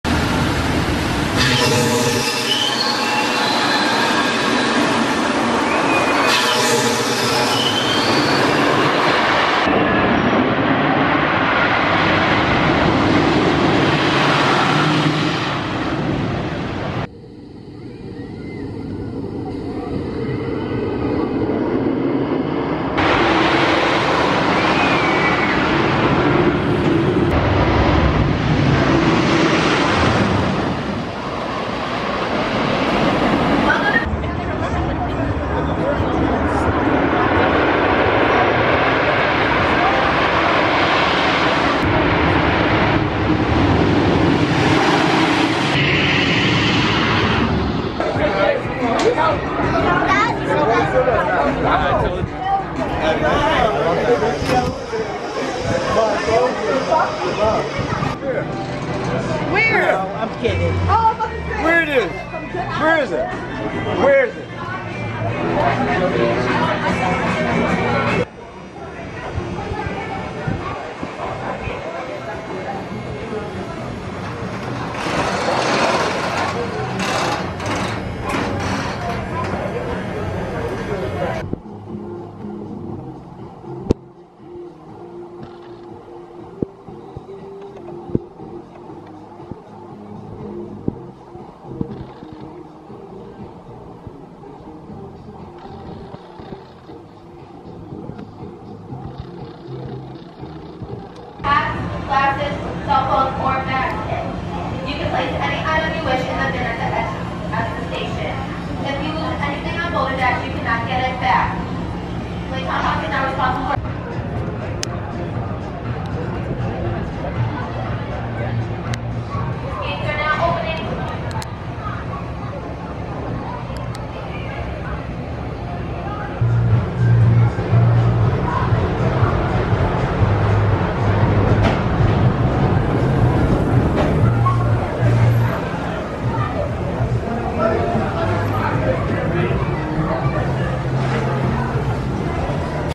Boulder Dash 2023 Off-Ride Footage | Titan Track, Tree Removal, & Speedy Pass | Non-Copyright
Boulder Dash at Lake Compounce has been voted annually as one of the world's best wooden roller coasters.  But there have been some big changes in 2023.  The ride received 580 feet of steel Titan Track, several trees have been removed, and a new ramp is being built into the station most likely for Speedy Pass.

Boulder Dash is an award-winning wood coaster from Custom Coasters International built on a wooded mountainside.  For footage of past years, please see the video below: